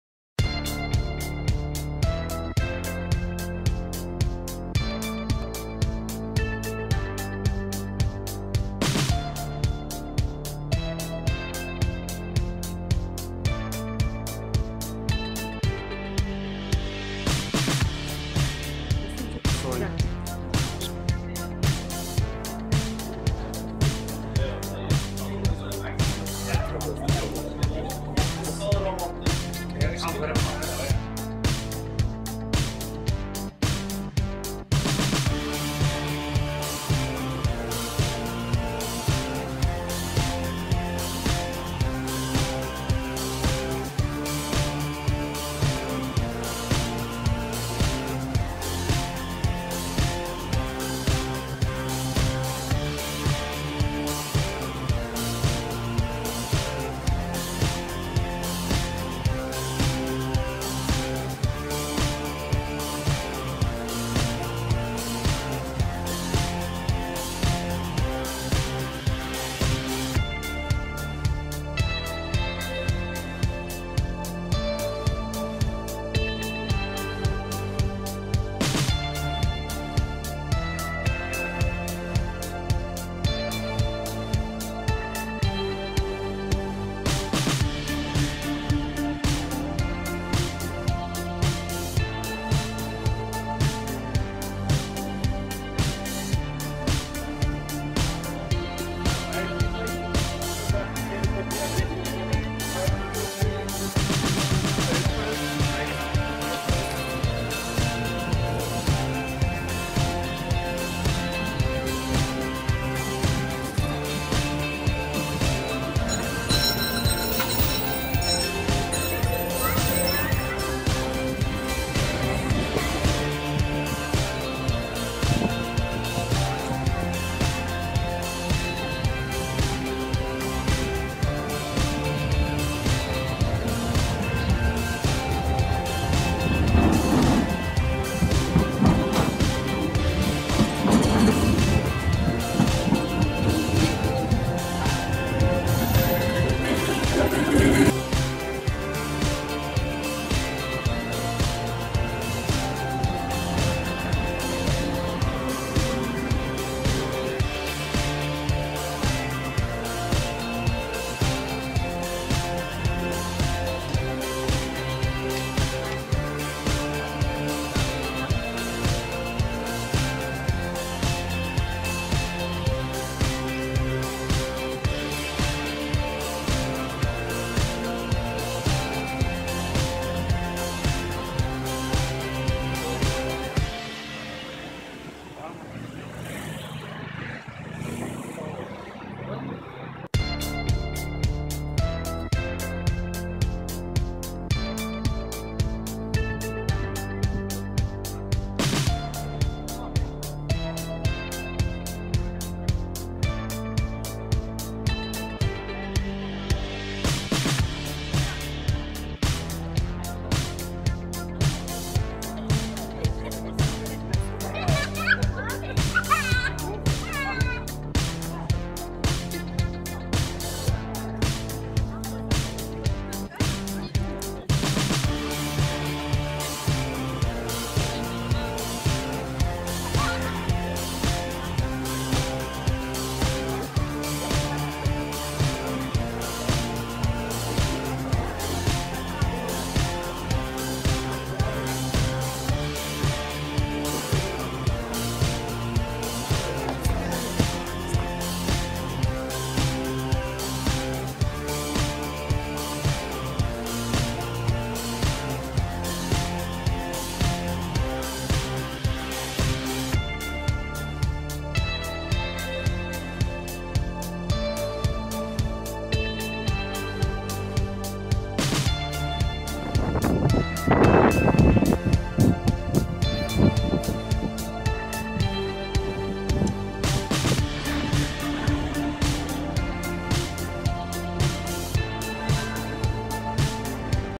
Brussels, Belgium; Must-See Attractions. Visit Brussels, 2018.
The Best of Visiting Brussels, Belgium, from the Manneken Pis Peeing Boy Statue to the Grand Place and to the Amazing Museums and Restaurants.